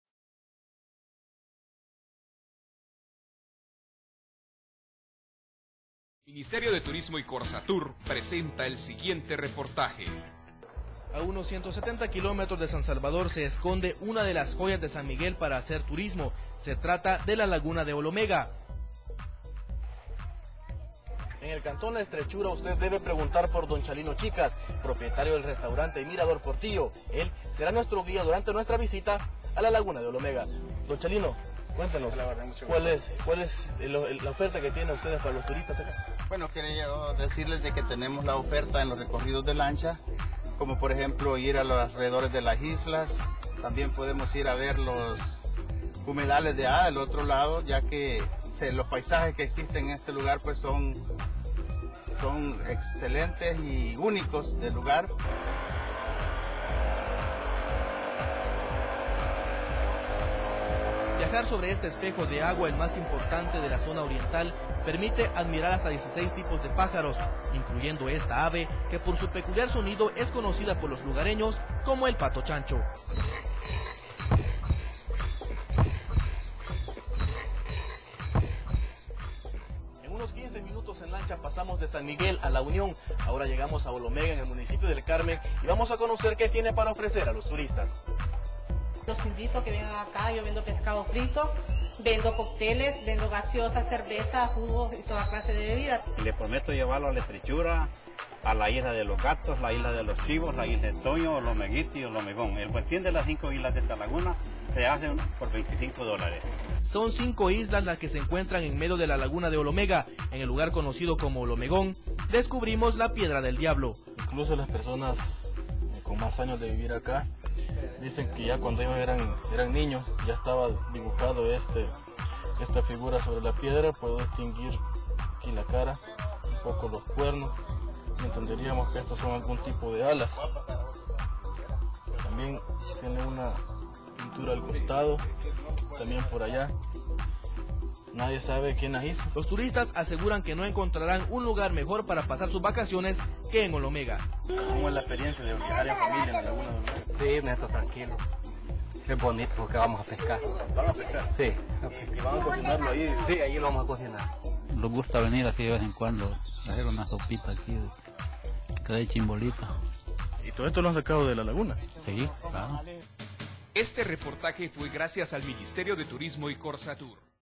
MITUR Olomega
Bajo el lema "Destinos de Verano", el Ministerio de Turismo de Honduras ha dado inicio a una campaña destinada a facilitar el acceso de turistas nacionales y extranjeros, a los principales lugares turísticos del país, con motivo de las vacaciones de Semana Santa.